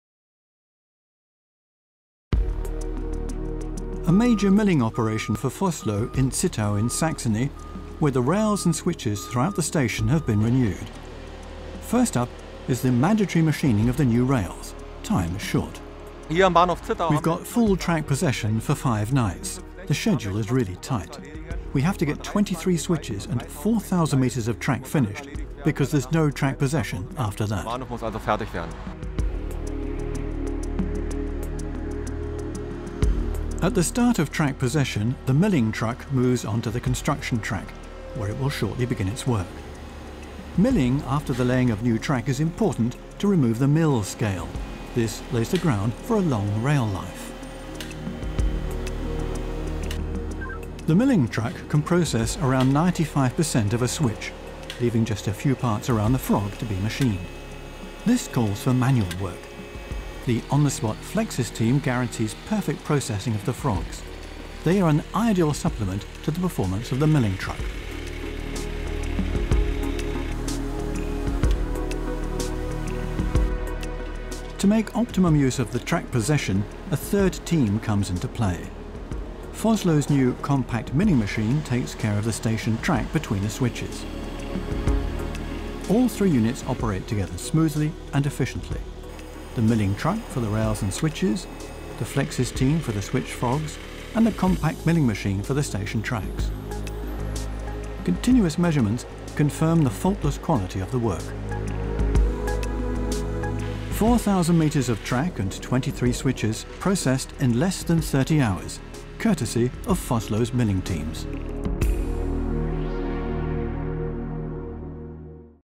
Vossloh Rail Services - Successful machining of newly-laid rails at Zittau railway station
Successful machining of newly-laid rails at Zittau railway station thanks to extensive switch-machining expertise: In the course of several night shifts in July involving closure of the entire station, Vossloh successfully completed the large-scale DB project of modernizing the Zittau railway station to make it a regional mobility hub. Over five night shifts and with a possession time of less than 30 hours, the team machined 23 turnouts, a double slip diamond crossing with inside points and more than 4,400 meters of rail as per the specifications in RIL 824.4010. The combination of road-rail milling truck and Flexis proved again to be an extremely efficient comprehensive package.